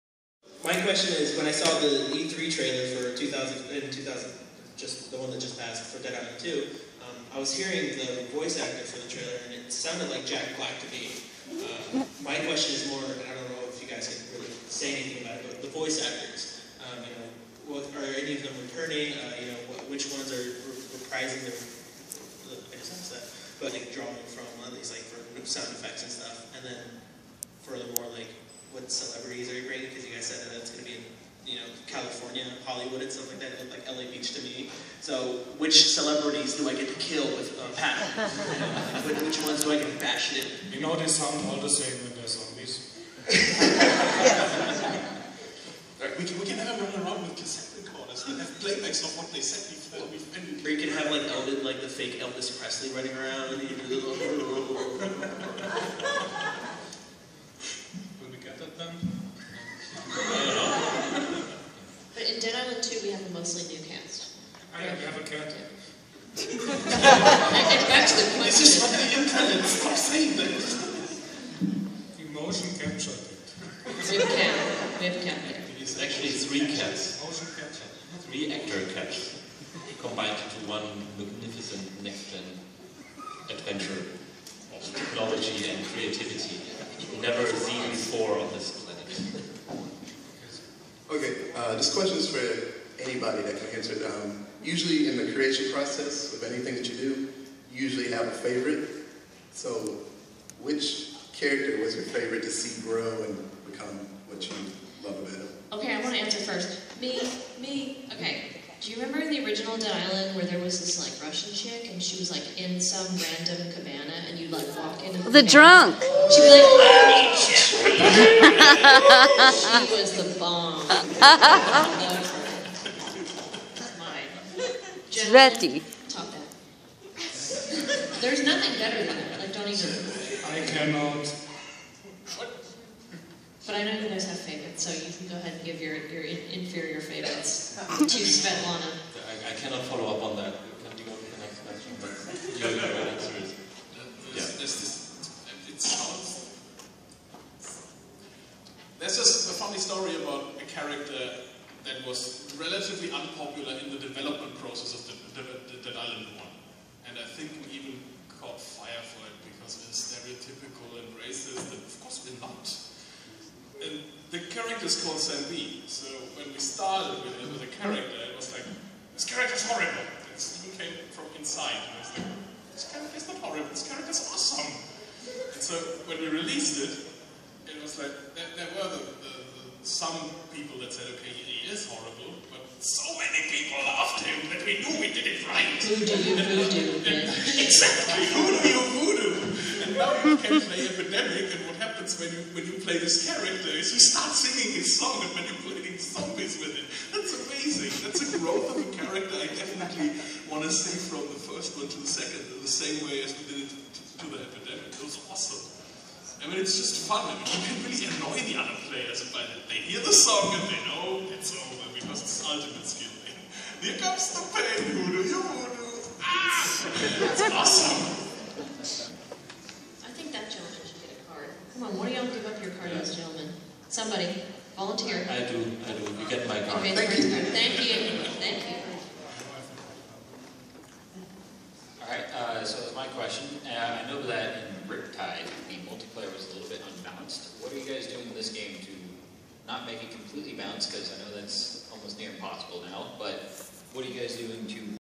"Dead Island 2" Panelsnippet, I
2014 San Diego Comic-Con International.  VERY amateur camera "work."